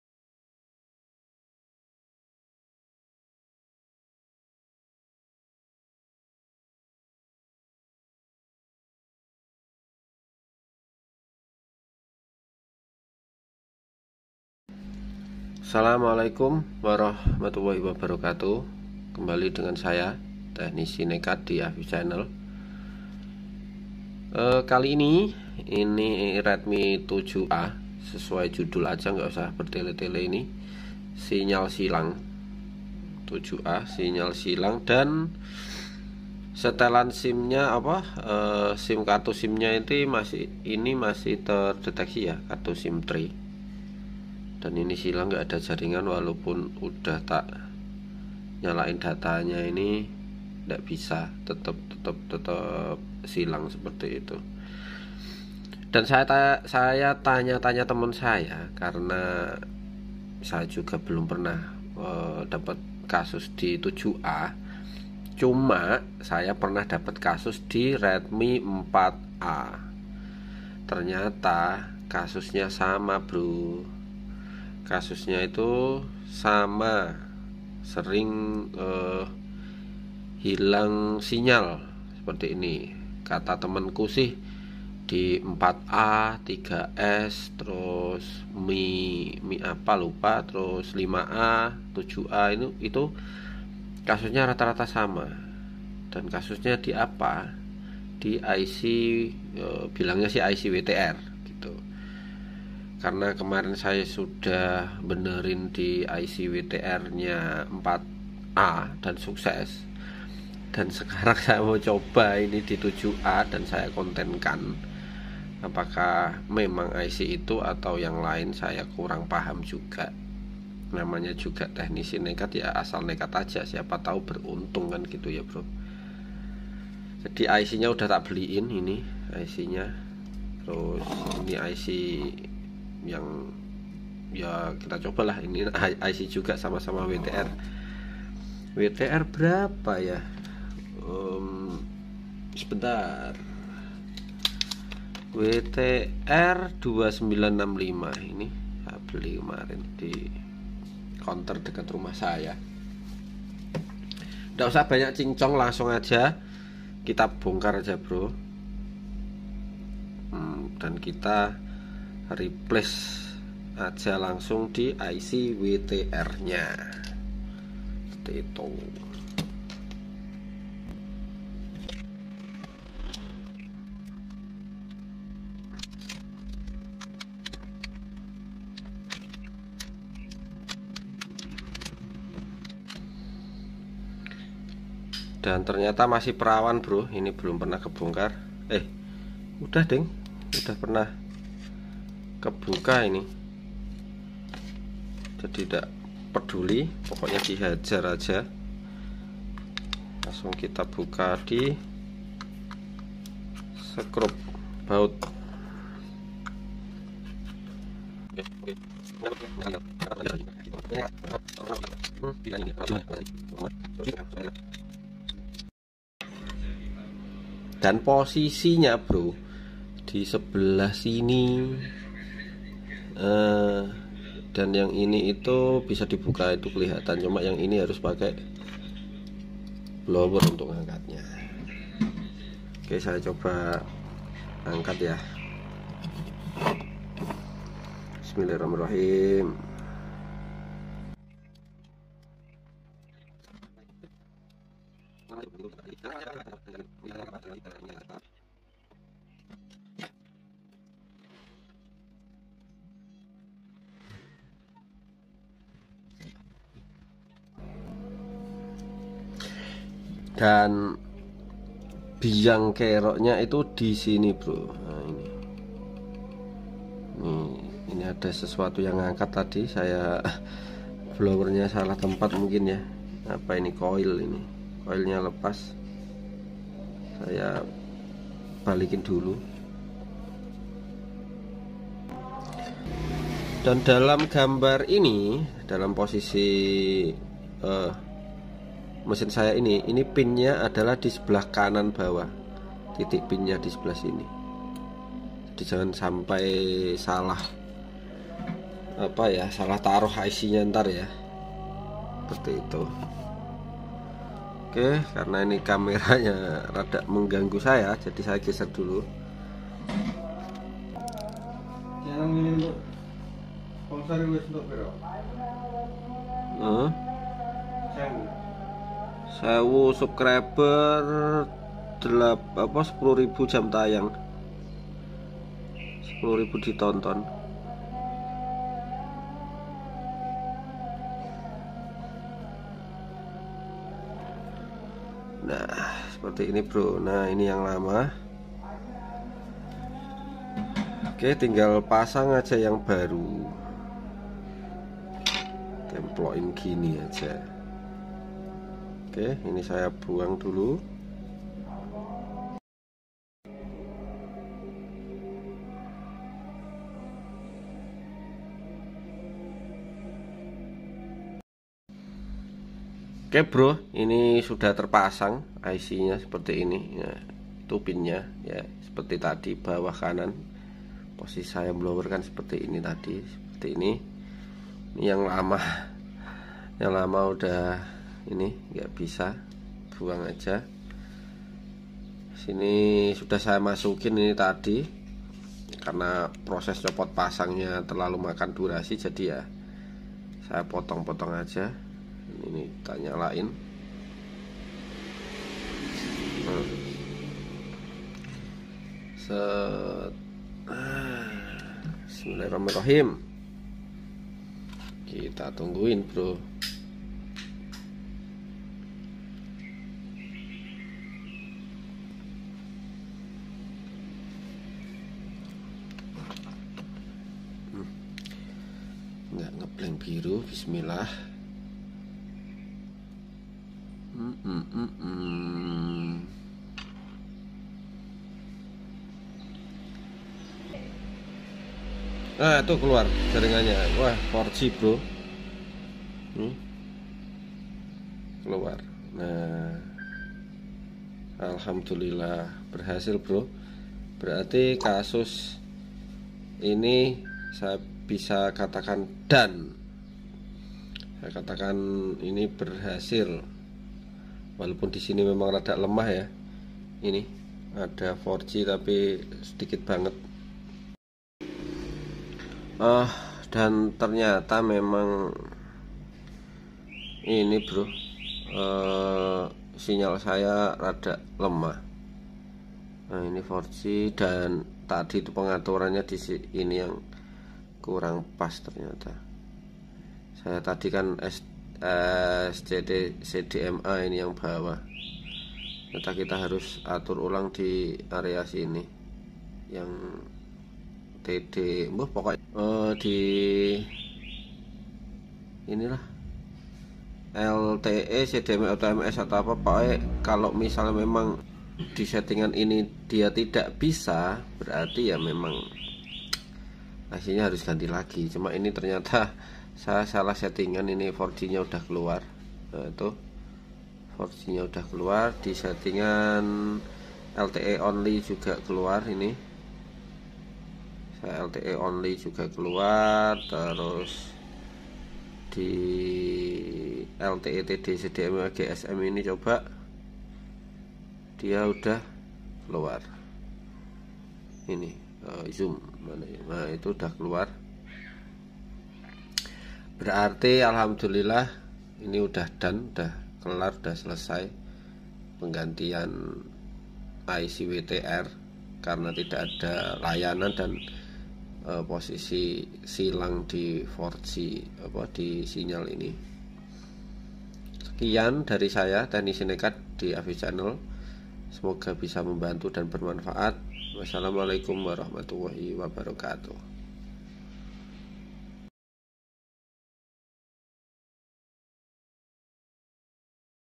Redmi 7a Sinyal Silang, Tidak Ada Layanan
Video perbaikan redmi 7a yg tidak ada layanan, sinyal hilang dan SIM card terdeteksi.

 Ini juga berlaku untuk Redmi 4a, 5a dan Redmi 3s

simak lengkapanya, semoga bermanfaat